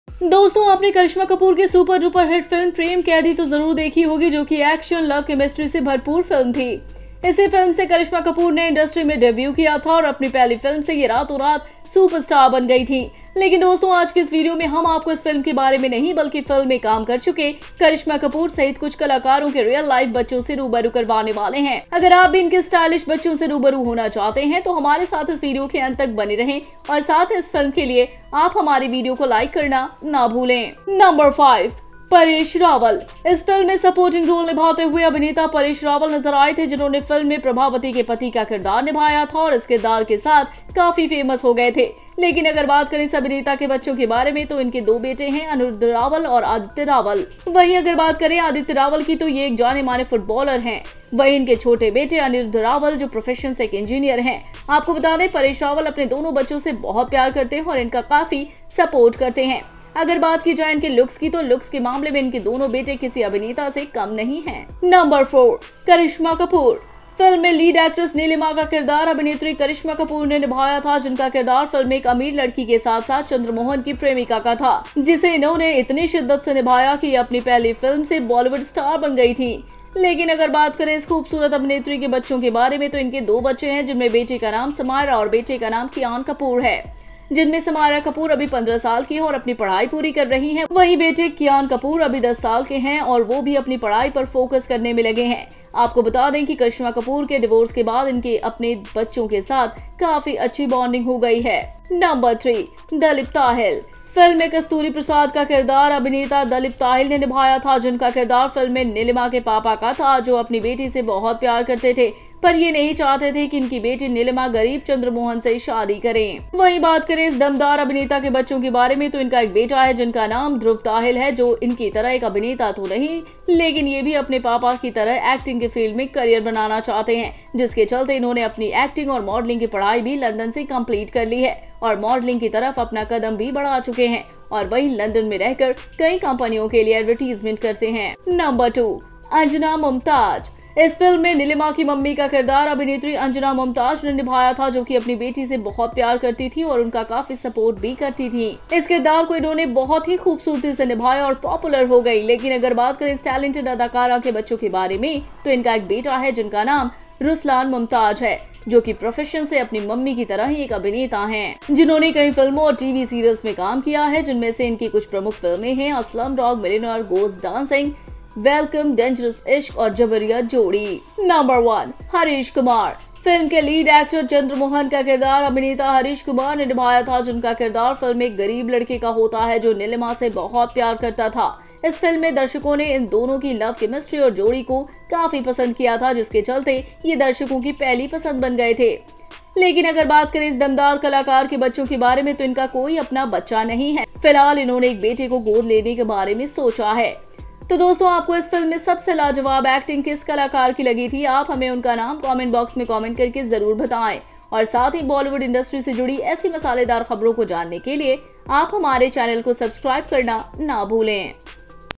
करिश्मा कपूर की फिल्म "कैदी" से फेमस हुए इन कलाकारों के ये है रियल लाइफ बच्चे Karishma Kapoor muvie
biography_Bollywood fact#

Title - Karisma Kapoor Kedi Movie Cast Child!

 Friends, you must have seen Karisma Kapoor's super duper hit film "Prem prisoner"!  Which was a film full of action love chemistry.

  Karisma Kapoor made her debut in the industry with this film and with her debut film, it became a superstar overnight.

 But friends, in this video of today, we will introduce you to real life children of some actors including Karisma Kapoor, who has worked in the film, not about this film.

 If you also want to interact with their stylish children, then stay with us till the end of this video and also you should not forget to like our video for this film.

 Number - 5 Paresh Rawal

 Actor Paresh Rawal was seen playing a supporting role in the film, who played Prabhavati's husband in the film and became quite famous with the character.  But if we talk about the children of this actor, then he has two sons, Anirudh Rawal and Aditya Rawal, if we talk about Aditya Rawal, then he is a well-known footballer.

 On the other hand, his younger son Anirudh Rawal, who is an engineer by profession, let me tell you that Paresh Rawal loves his two children very much and also supports them a lot,

 If we talk about his looks, then both of his sons are no less than any actor in terms of looks.

 Number - 4 Karisma Kapoor

 In the film, the lead actress Neelima was played by actress Karishma Kapoor, who played a rich girl in the film as well as Chandramohan's girlfriend whom she played so strongly that she became a Bollywood star from her first film.

  But if we talk about the children of this beautiful actress, then she has two children, among whom the daughter's name is Adhara and the son's name is Kian Kapoor.  In which Adabra Kapoor is still 15 years old and is completing her studies.  At the same time, his son Kian Kapoor is still 10 years old and he is also focusing on his studies.  After the Divorce of Karisma Kapoor, she has got a very good bond with her children.

 Number - 3 Dalip Tahil

 In the film, Kasturi Prasad was played by actor Dalip Tahil, whose character in the film was Neelima's father who loved his daughter very much but did not want her daughter Neelima
 Marry poor Chandramohan.

 If we talk about the children of this powerful actor, then he has a son named Dhruv Tahil.  Who is not an actor like him but also wants to make a career in the field of acting like his father, due to which he has completed his acting and modeling studies from London and has also made his step towards modeling and there.  Advertisements for many companies based in London.

 Number - 2 Anjana Mumtaz

 In this film, Neelima's mummy was played by actress Anjana Mumtaz, who loved her daughter very much and also supported her, she played this character very beautifully and became popular.  But if we talk about this talented actress' children then she has a son whose name
 Ruslan is Mumtaz who is an actor from the profession just like his mummy who has worked in many films and TV serials, some of which are major films like "A Slumdog Millionaire Goes Dancing", "Welcome", "Dangerous Ishq" and "  Forced pair ".

 Number - 1 Harish Kumar

 The film's lead actor Chandramohan was played by actor Harish Kumar, whose character in the film is that of a poor boy who loved Nilima, in this film, the audience loved the duo's love chemistry and the pair, due to which it  Became the first choice of the audience.

 But if we talk about the children of this powerful artist, then he has no child of his own, at the moment he has thought about adopting a son.

 So friends, what was the most amazing acting artist in this film, you must tell us their name by commenting in the comment box and you should not forget to subscribe to our channel to know such spicy news related to Bollywood industry.